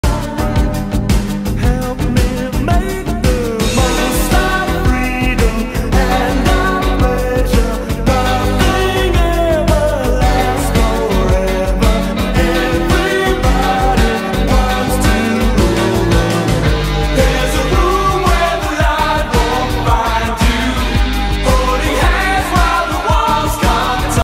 baustelle maurer handwerk bricklayer constructionsite girl fun shorts tutorial work job
Fenstersturz einschalen leicht gemacht🫱🏼‍🫲🏾

Hier kommen noch 6 cm Dämmung drauf deswegen sitz der Sturz niedriger